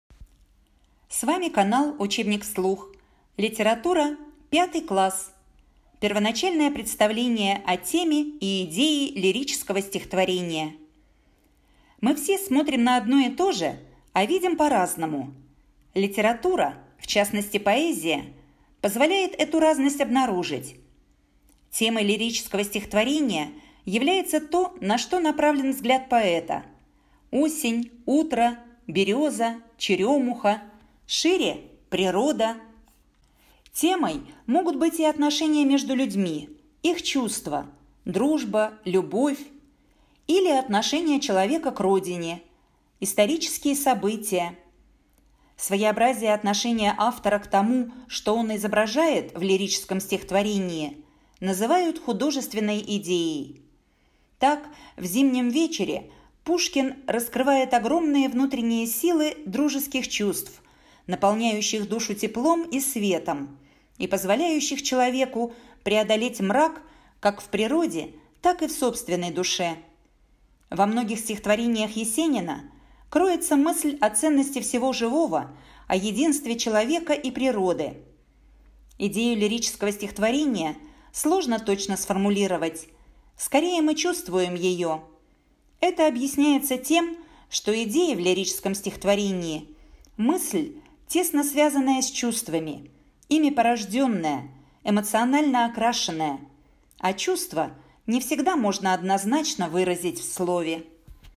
Первоначальное представление о теме и идее лирического стихотворения. Литература 5 класс.
Первоначальное представление о теме и идее лирического стихотворения. Литература 5 класс ч.1, под редакцией Сухих.

"Учебник вслух" - это сборники уроков и произведений из школьных учебников.